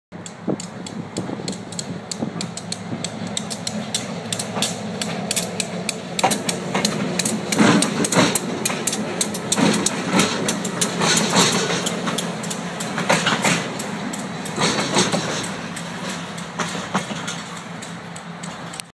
Pressing Quarters
Passed a train on my way home for lunch and had some change in the car.  If you strain a little bit, you can see the coins bounce about at the train wheels pass over them.